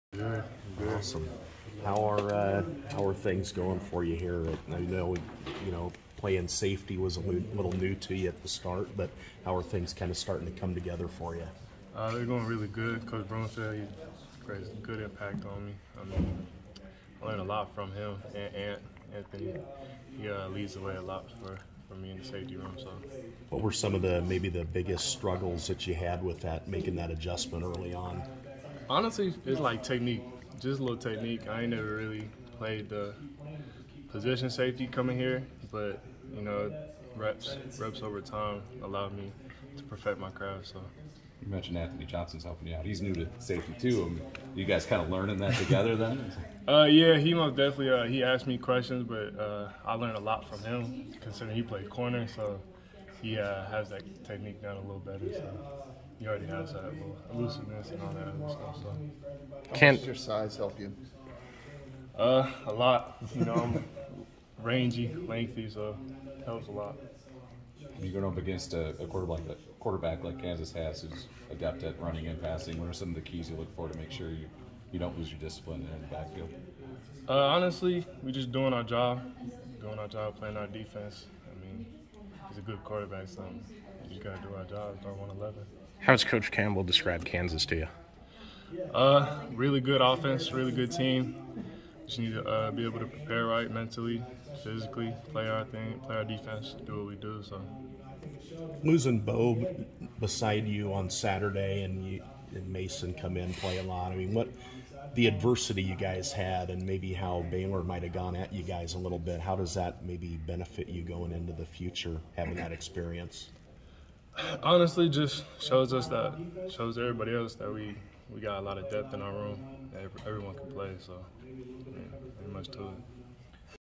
CFTV: Malik Verdon talks season so far, Kansas preparation
Iowa State's Malik Verdon spoke to media members Tuesday at the Stark Performance Center, ahead of Saturday's matchup with Kansas.